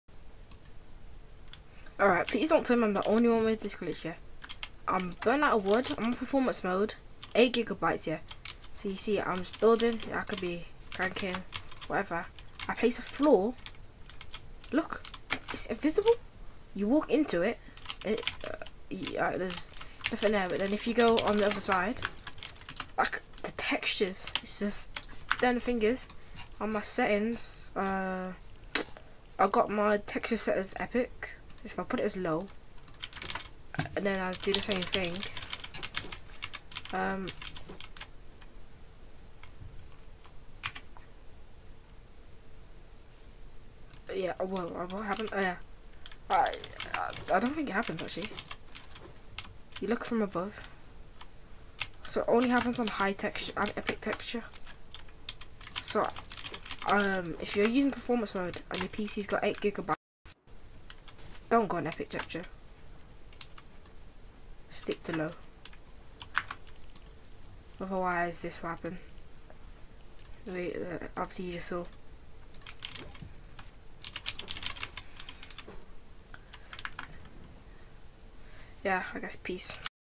Performance Mode Invisible Build Glitch: Texture Bug And How To Fix It! (Read Description) (8gb Ram)
This Glitch Will Most Likely Happen To You If Your PC Has 8gb Of Ram Or Below.

If You Are Having This Glitch/Problem The Only Way To Fix It Is By Keeping Your Texture On Low Or Medium.